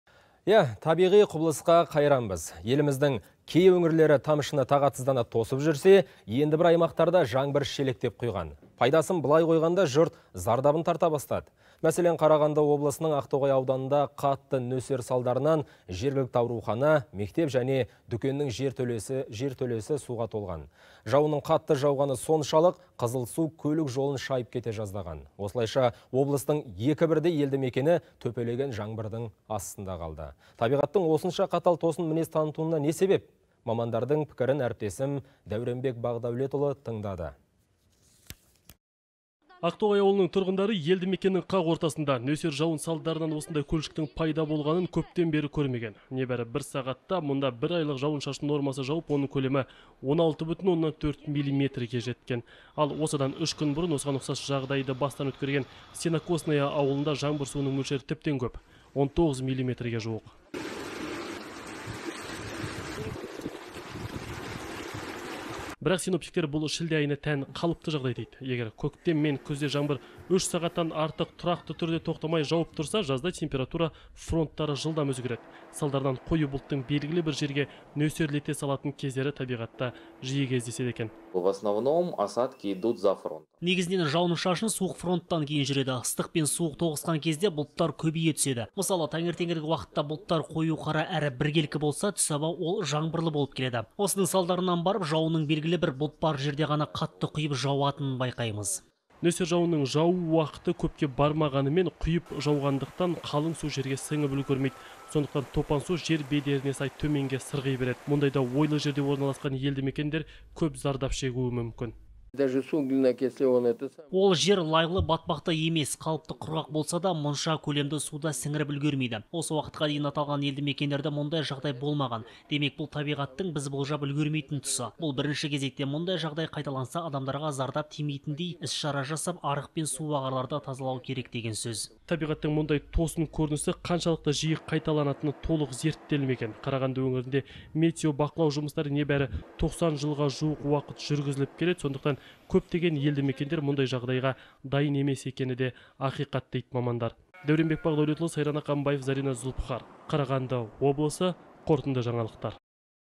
Қарағанды облысының Ақтоғай ауданында қатты нөсер салдарынан жертөлелер суға толған
Еліміздің кей өңірлері тамшыны тағатсыздана тосып жүрсе, енді бір аймақтарда жаңбыр шелектеп құйған.

Пайдасын былай қойғанда жұрт зардабын тарта бастады. Мәселен, Қарағанды облысының Ақтоғай ауданында қатты нөсер салдарынан жергілікті аурухана, мектеп және дүкеннің жертөлесі суға толған. Қазір мамандар жағдай қалыпты, қызыл судың тысқа шығарылғанын айтып отыр.